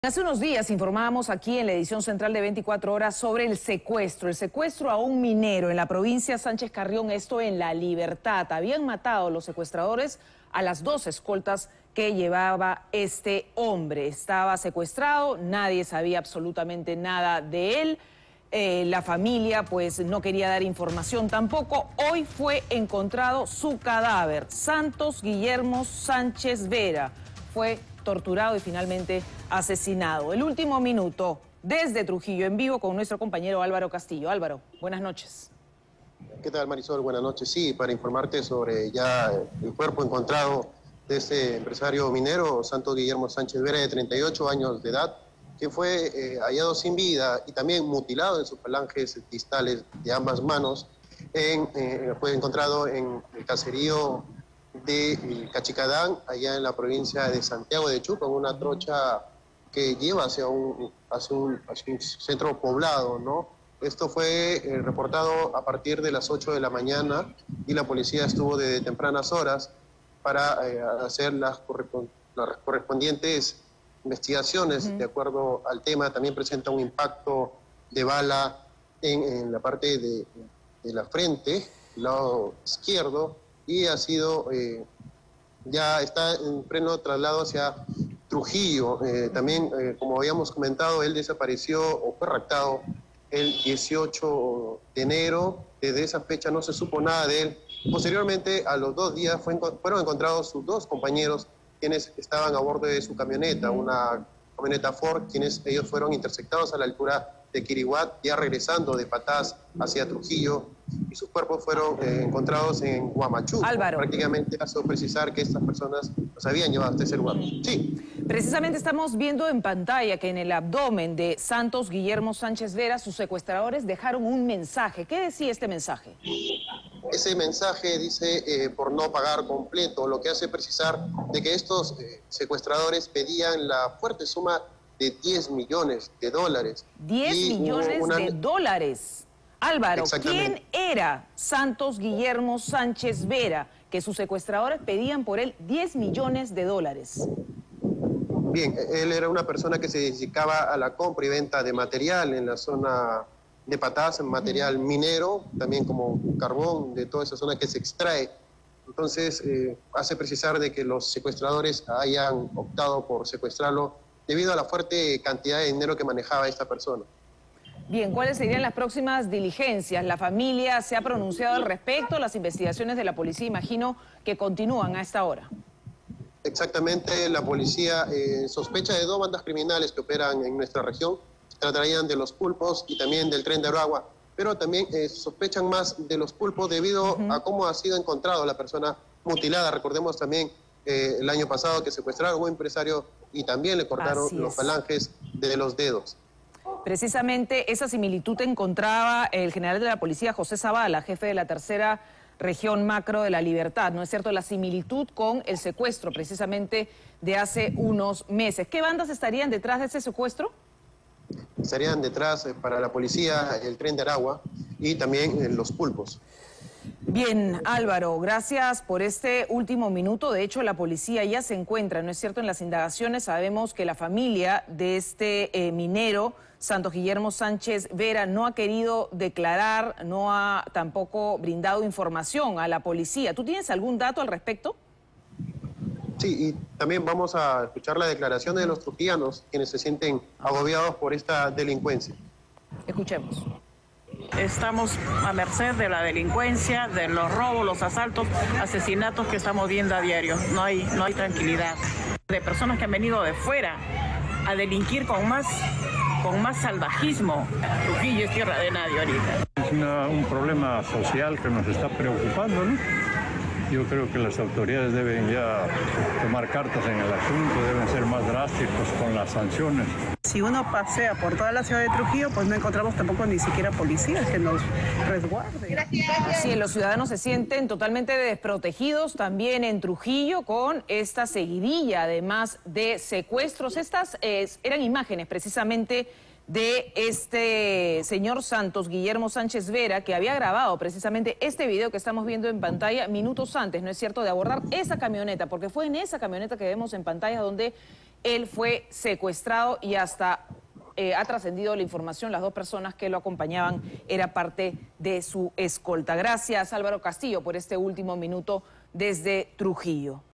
Trujillo: hallan cadáver de empresario minero que fue secuestrado hace 6 días
En un descampado de la provincia de Huamachuco fue hallado el cadáver del empresario minero Santos Guillermo Sánchez Vera, quien fue secuestrado hace seis días cuando viajaba desde Pataz hacia Trujillo junto a sus dos trabajadores.

El cuerpo de la víctima estaba mutilado y sus verdugos escribieron un mensaje que a la letra dice: “Por no pagar completo”. No se descarta que la organización “Tren de Aragua” y “Los Pulpos” estén detrás de este macabro asesinato.

Según se conoció, los secuestradores exigían a sus familiares el pago de 10 millones de dólares por su liberación, monto que finalmente no habría sido pagado en su totalidad. Peritos de criminalística iniciaron las investigaciones.

MACABRO HALLAZGO

Hay que señalar que a inicios de semana, agentes de la Policía Nacional hallaron los cuerpos de sus dos trabajadores, Roger Paredes Aguilar y Edgar Vega Torres, en el sector de La Ramada. Ambos también presentaban signos de tortura.